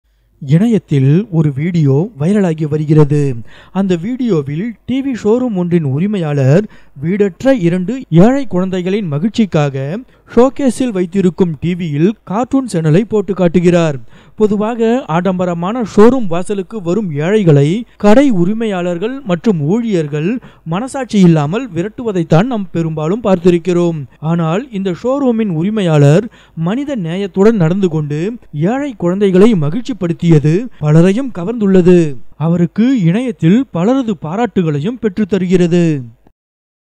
டிவி ஷோரூம் உரிமையாளர் செய்த நெகிழ்ச்சியான செயல்! குவியும் பாராட்டுக்கள்
This video shows an act of humanity of a TV showroom owner..